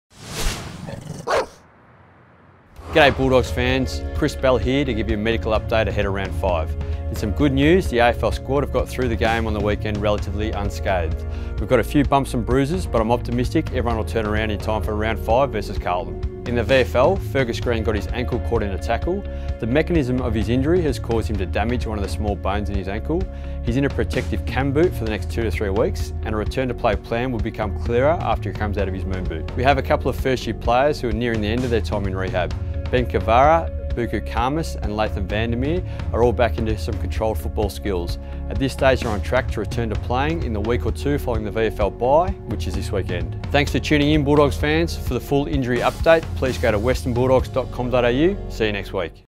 Medical Room | Latest on Greene, Cavarra, Khamis and Vandermeer
Chris Bell gives the medical room update on Fergus Greene, Ben Cavarra, Buku Khamis and Laitham Vandermeer